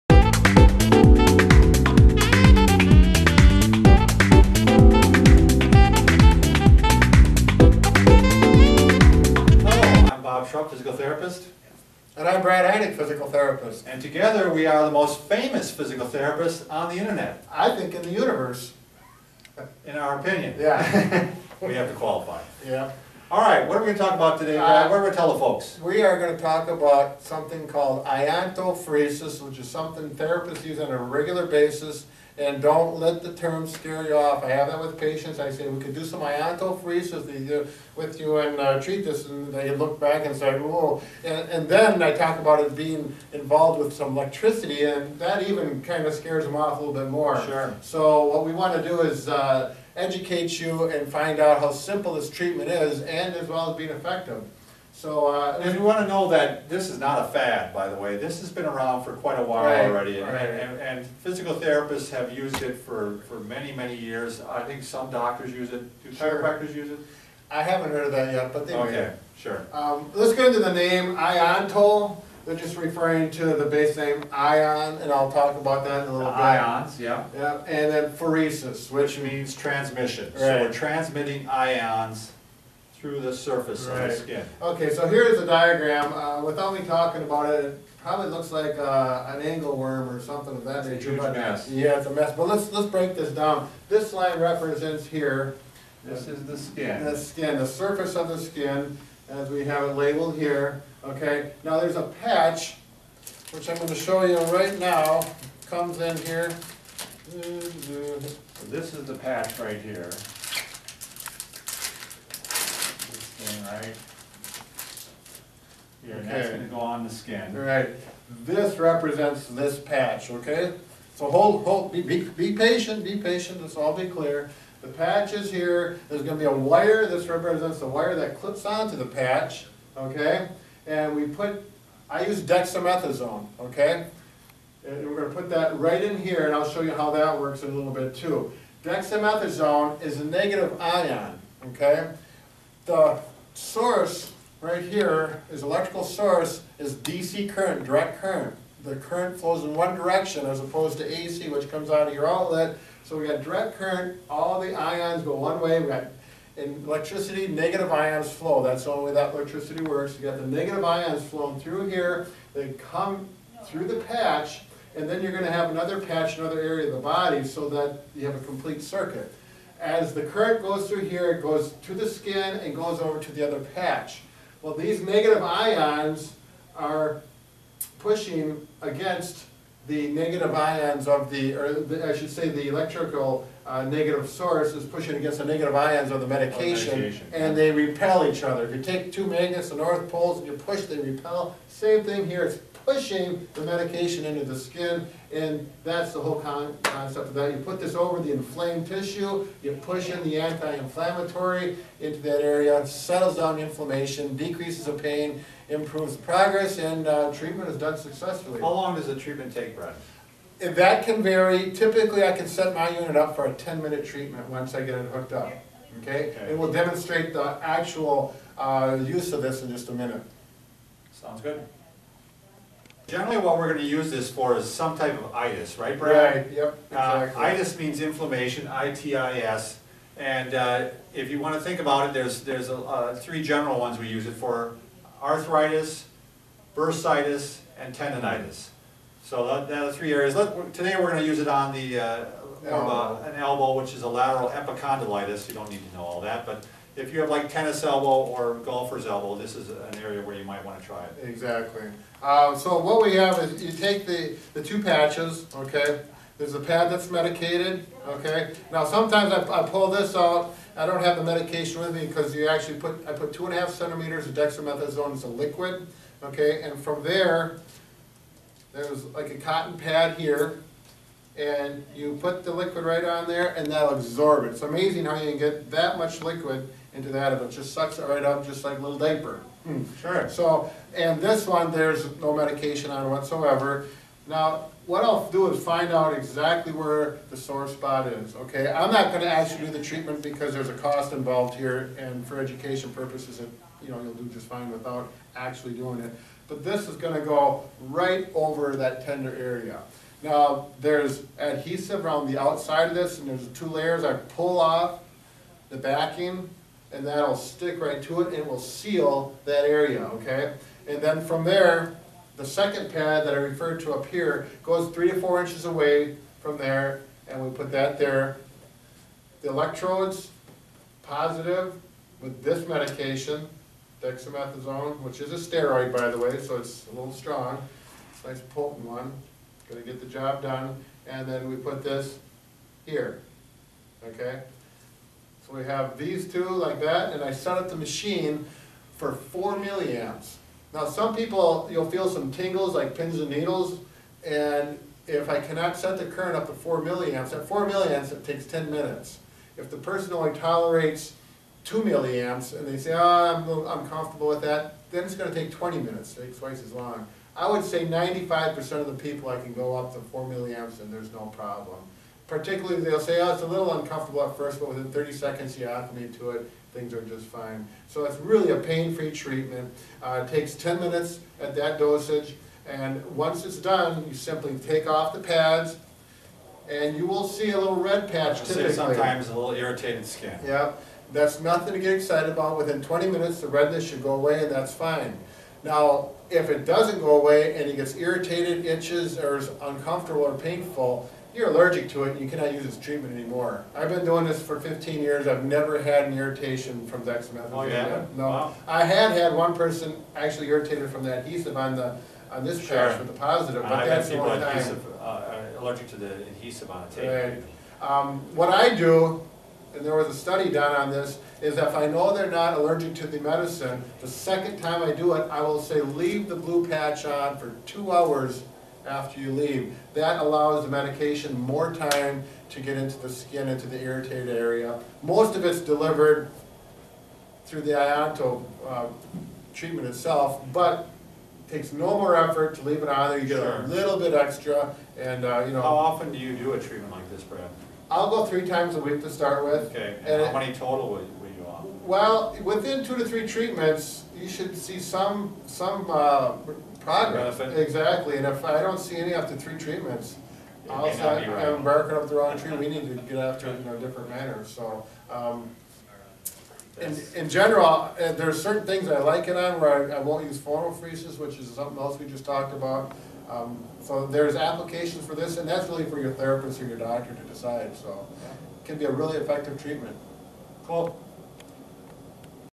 Iontophoresis: for tennis elbow or golfer's elbow
"Famous" physical therapists Bob Schrupp and Brad Heineck describe how iontophoresis works and how it can be used on tennis elbow or golfer's elbow (elbow pain).